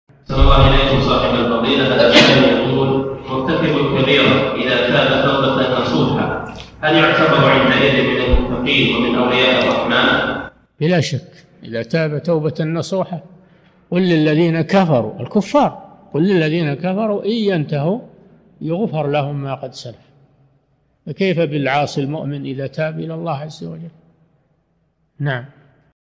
[22 -201] مرتكب الكبيرة إذا تاب توبة نصوحا هل يعتبر حينئذ من المؤمنين؟ - الشيخ صالح الفوزان
-[22 -201] السؤال: مرتكب الكبيرة إذا تاب توبة نصوحا هل يعتبر حينئذ من المؤمنين ومن أولياء الرحمن؟
الجواب: بلا شك إذا تاب توبةً نصوحًا،  ﴿قُلْ لِلَّذِينَ كَفَرُوا﴾ وهم كفار ﴿قُلْ لِلَّذِينَ كَفَرُوا إِنْ يَنتَهُوا يُغْفَرْ لَهُمْ مَا قَدْ سَلَفَ﴾ [الأنفال:38]، فكيف بالعاصي المؤمن إذا تاب إلى الله عز وجل؟
للاشتراك في قناة الشيخ صالح الفوزان

للاستفسار والإقتراحات والدعم
رئيس الفريق
لمشروع كبار العلماء